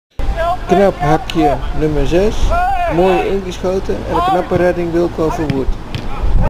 Amazing save Wilco Verwoerd | voetbal football hakje save goalkeeper .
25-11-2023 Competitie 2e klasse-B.
VVA Achterberg - vv de Merino's.
Eerst knap hakje van de nr. 6 vv de Merino's Roy Terschegget, gevaarlijke voorzet, en een knappe redding van Wilco Verwoerd VVA Achterberg.